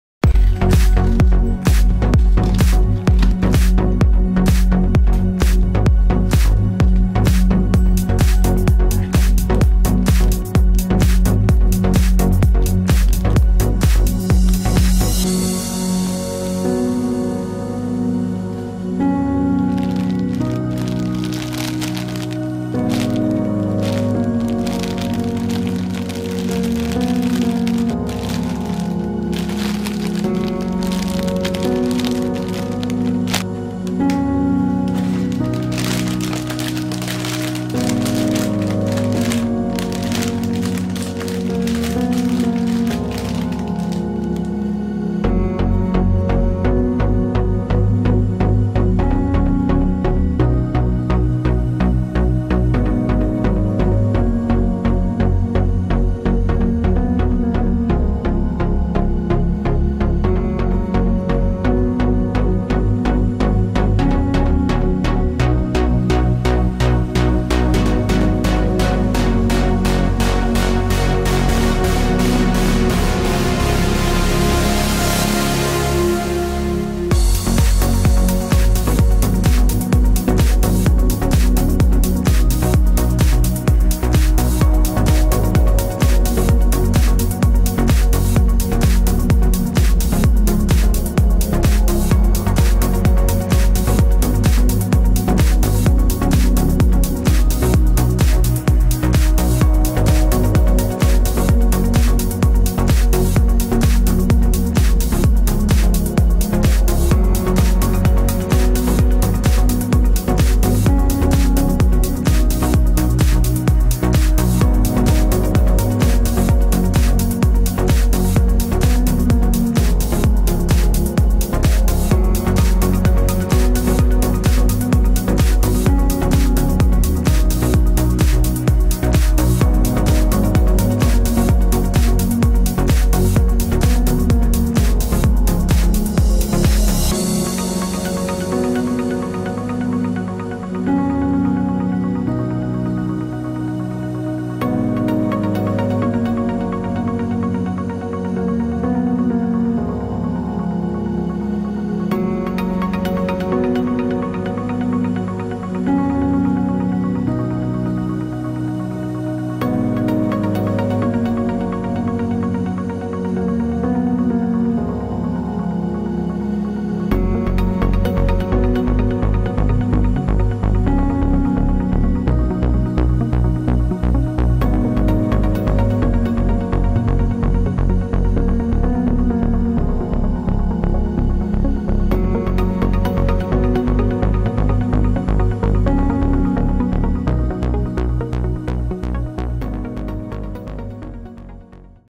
Bubble Bobble Peb (Kululun) Pab (Kororon) Plush Unboxing
Unboxing of Bubble Bobble Peb (Kululun) and Pab (Cororon) Plush Dragons. Content in this video is intended for a general audience.